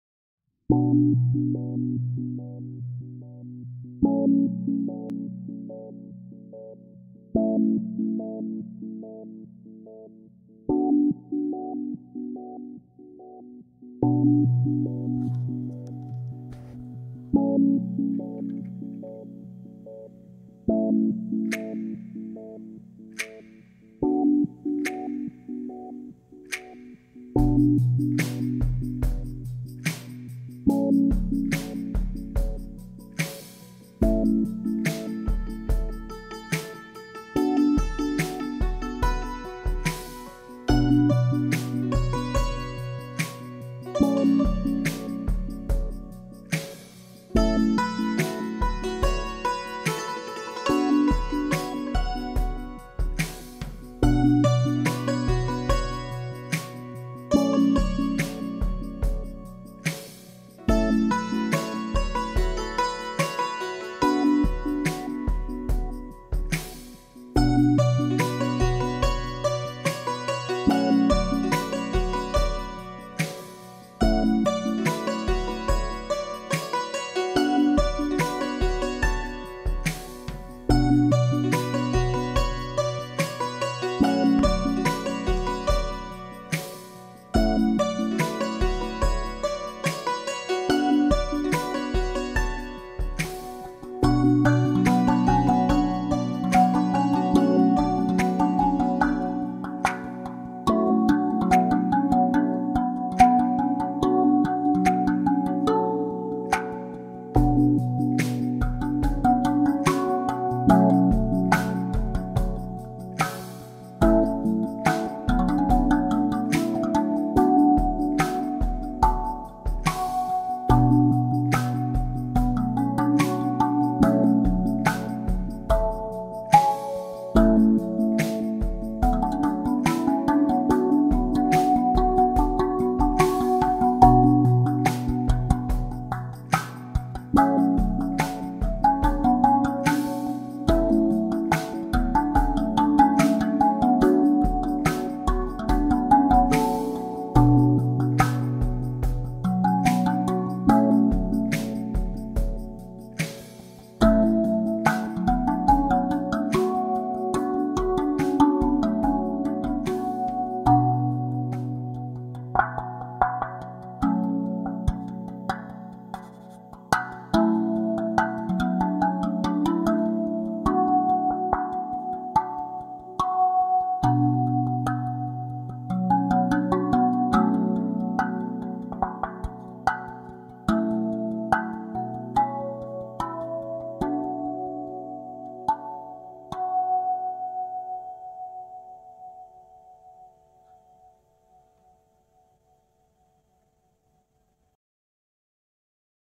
Dmitry Soul - Live-Looping with electronic beats, santoor and rav vast.
A small live looping session by Dmitry Soul recorded in Ubud, Bali.

The instruments are rav vast and Iranian santur.